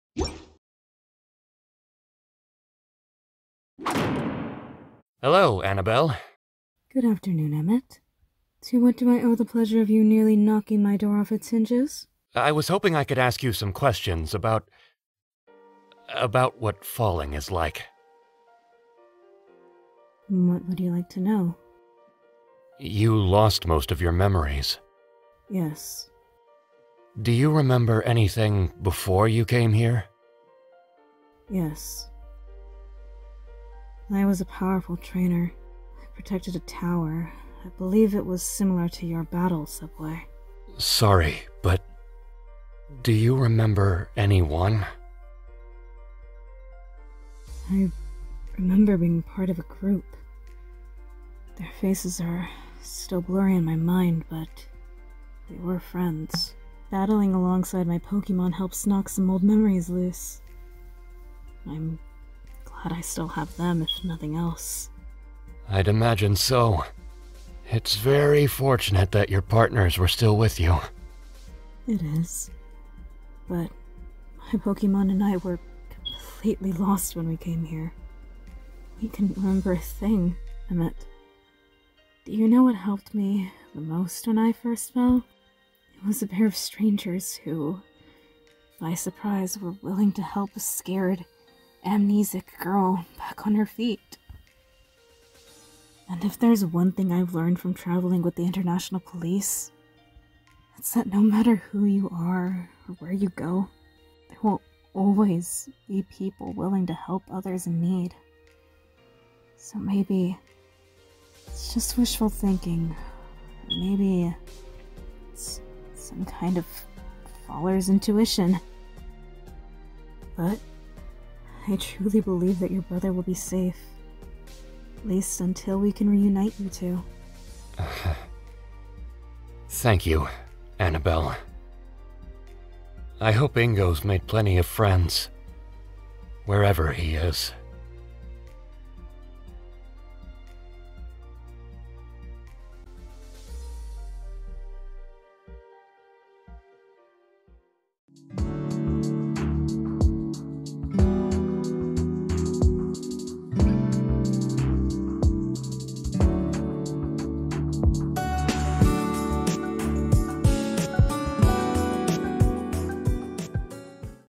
Falling Memories (Pokemon Comic Dub)
Here's the creator are the comic.

Voice Cast:
@MelodySketch327 as Anabel